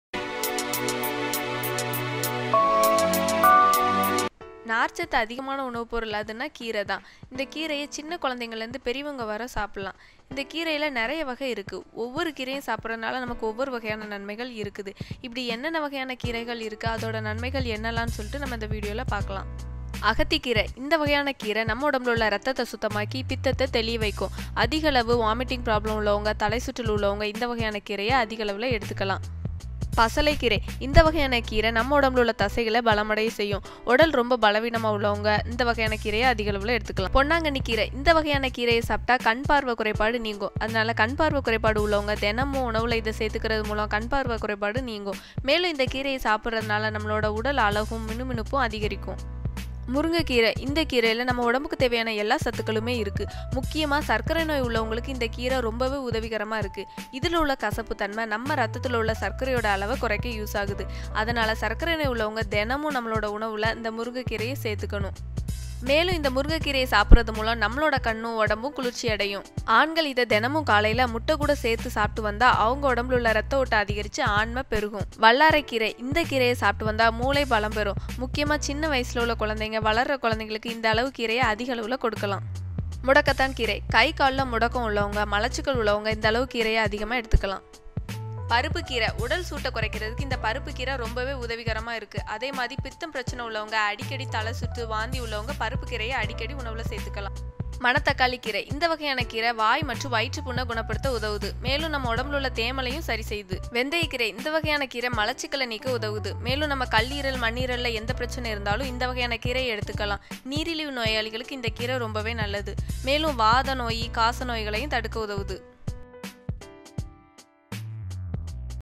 Health benefits of Moringa leaf for all Disease cure
This video will tell u about different types of green leaves and its medical benefits
agathi keerai
ponnanganni keerai
paruppu keerai
vendhaya keerai
manathakkali keerai
pasalai keerai
murungai keerai
vallarai keerai
mudukathan keerai